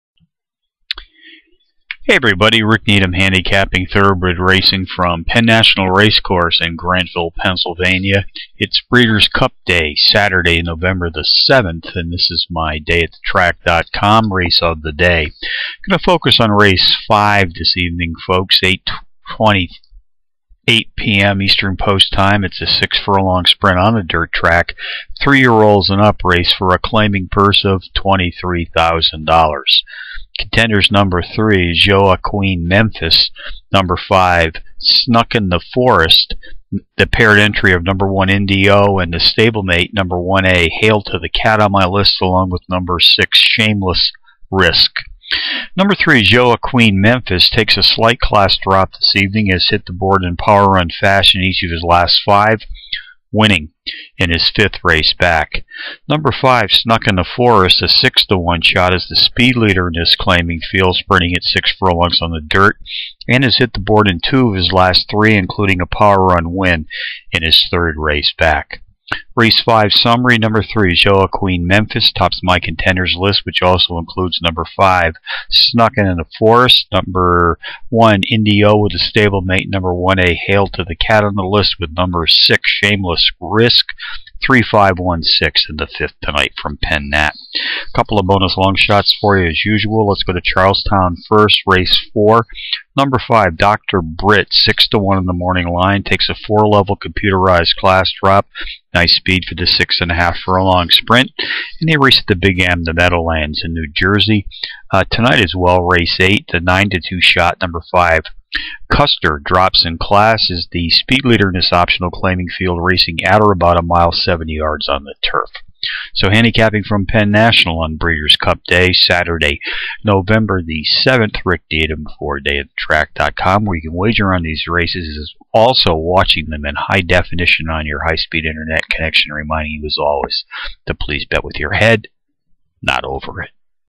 pennnat
Race #5 - Penn National - Saturday November 7, 2009 + bonus longshots from Charles Town & The Meadowlands